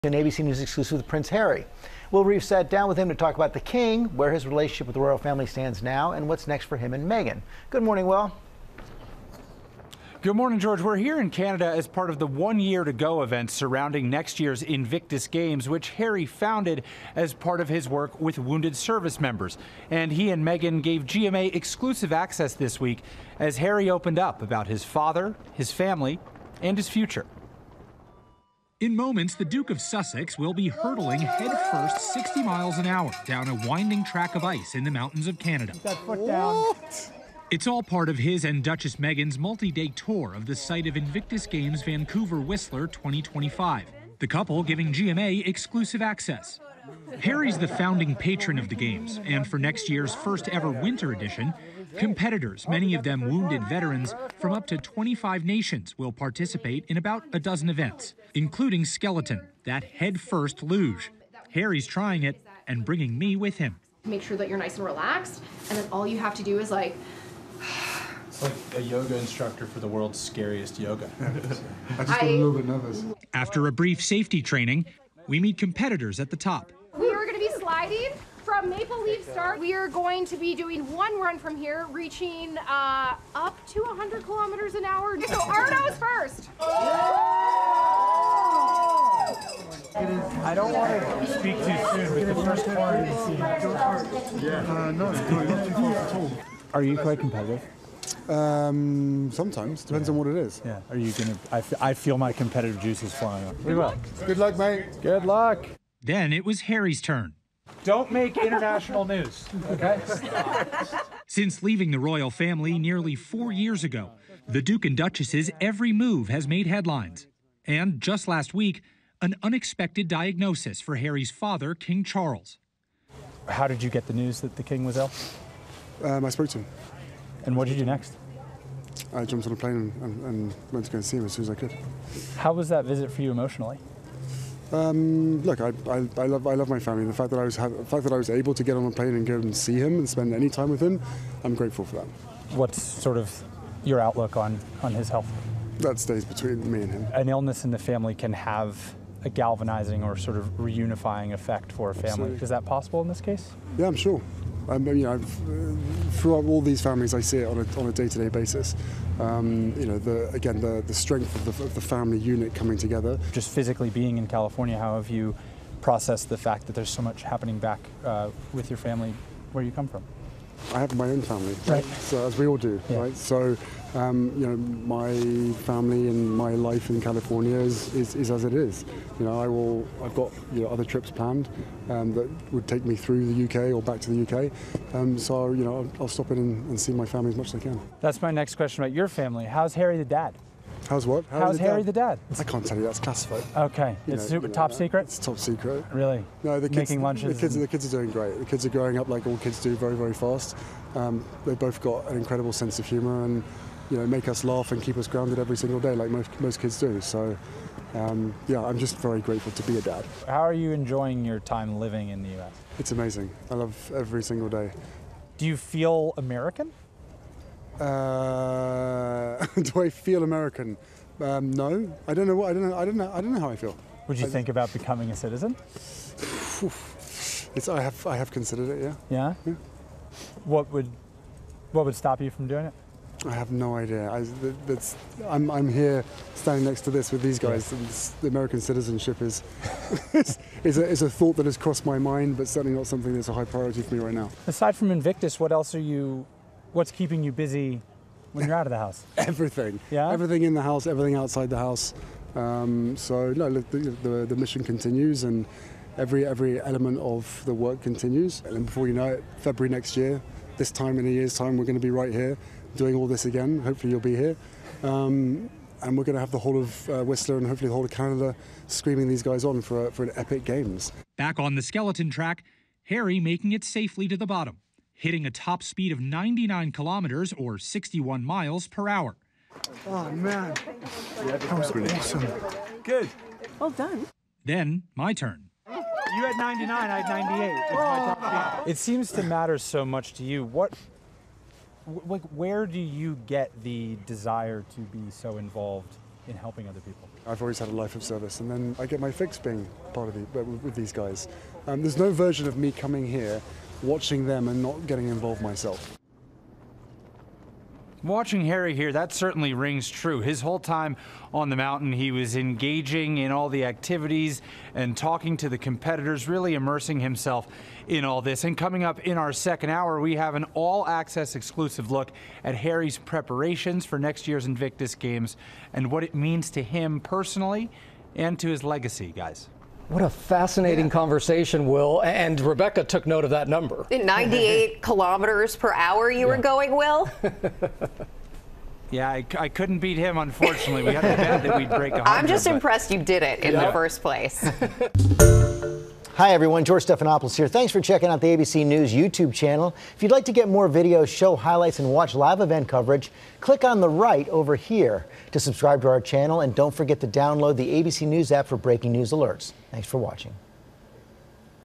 Prince Harry talks seeing King Charles amid cancer battle: ‘I love my family’
The Duke of Sussex tells ABC News’ Will Reeve he was “grateful” he was able “to get on a plane and go see” his father following the king’s diagnosis.